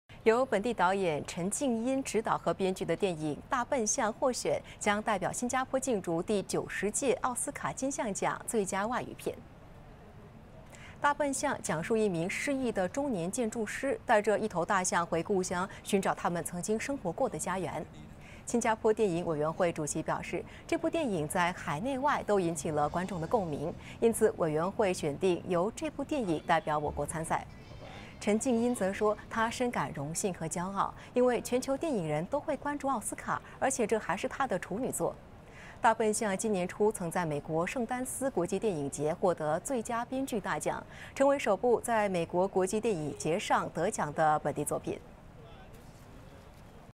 本地电影《大笨象》获选代表我国 报名明年奥斯卡金像奖
由本地导演陈敬音执导和编剧的电影《大笨象》获选，将代表新加坡竞逐第90届奥斯卡金像奖最佳外语片。